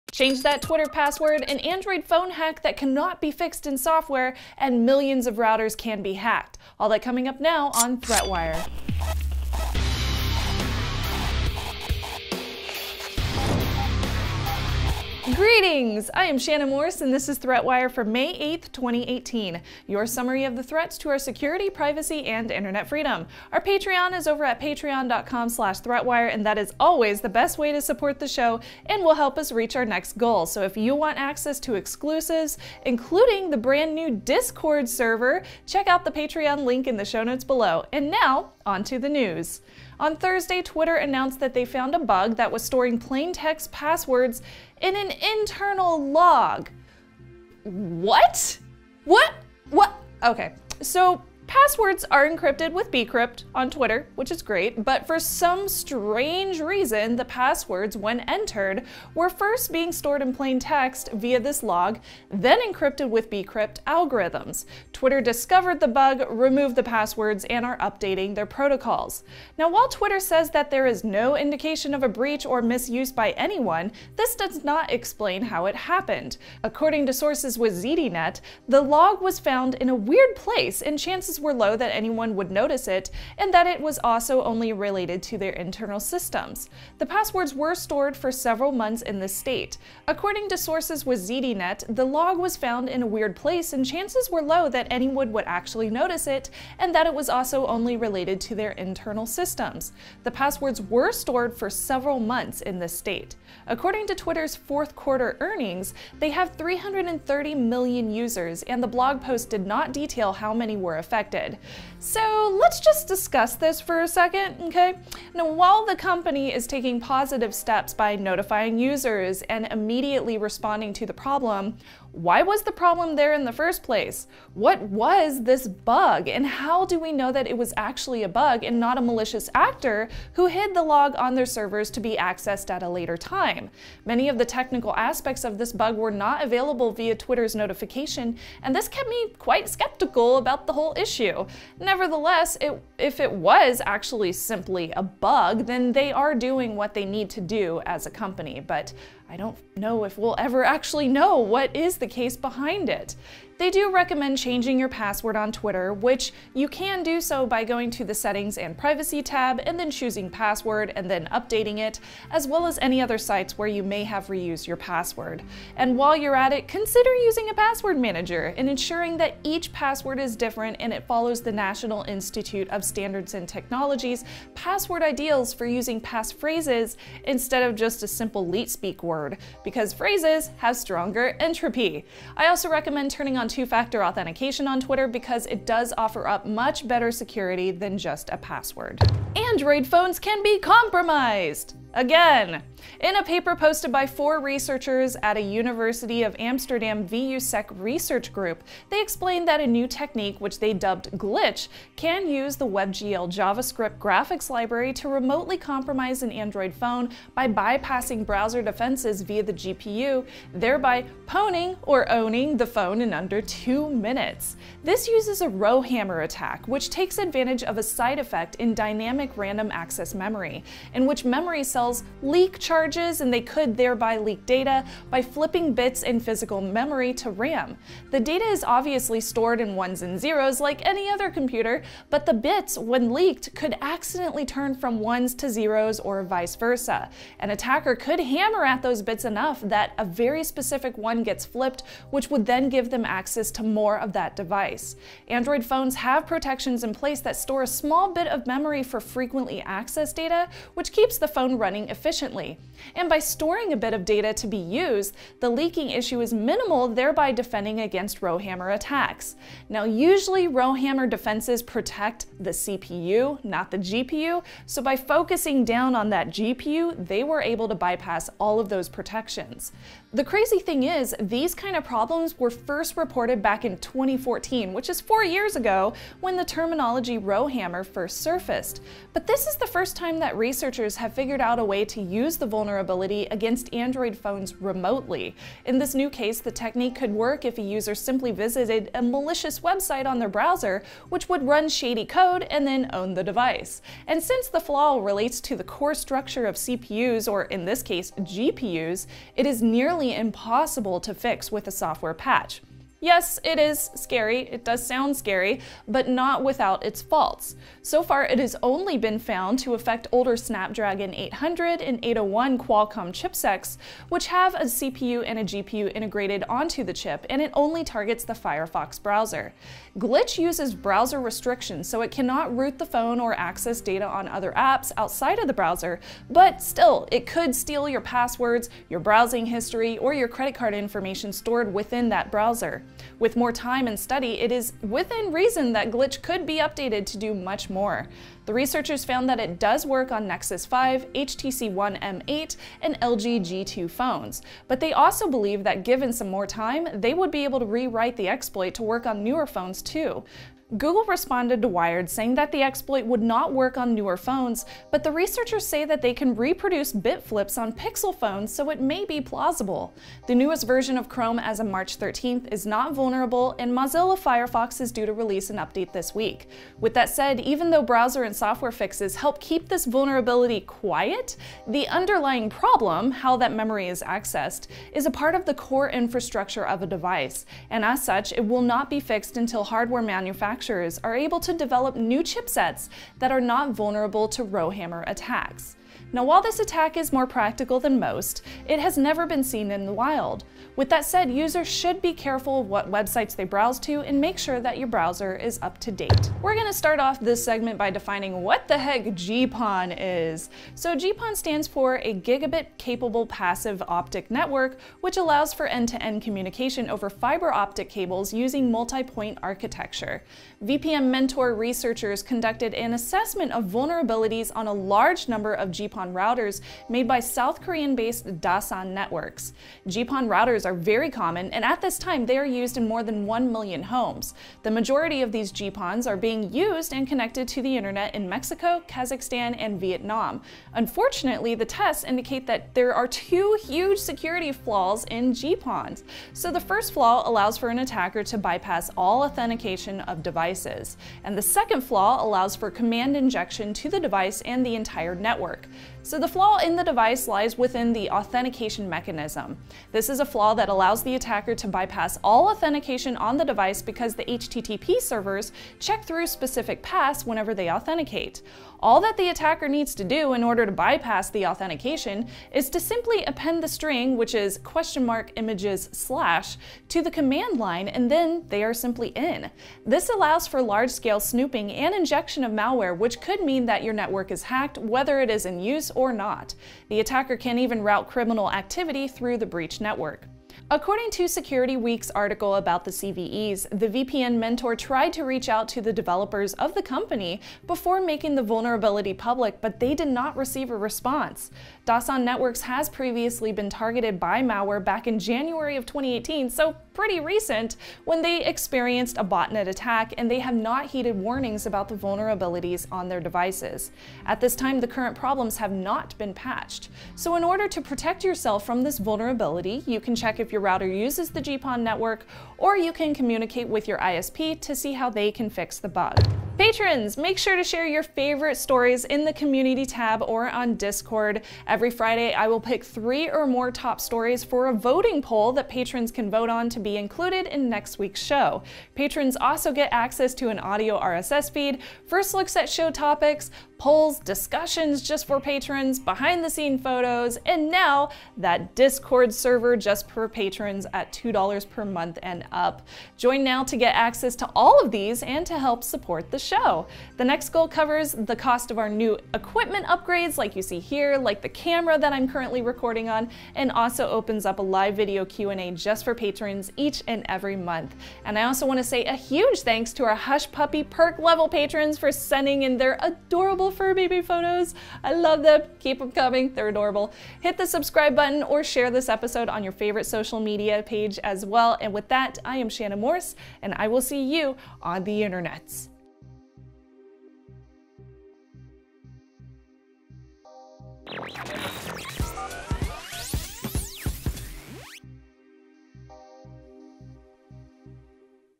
An Unfixable Android Hardware Issue - ThreatWire
Hak5 -- Cyber Security Education, Inspiration, News & Community since 2005:
Change that twitter password, an Android phone hack that can’t be fixed in software, and millions of routers can be hacked! All that coming up now on ThreatWire.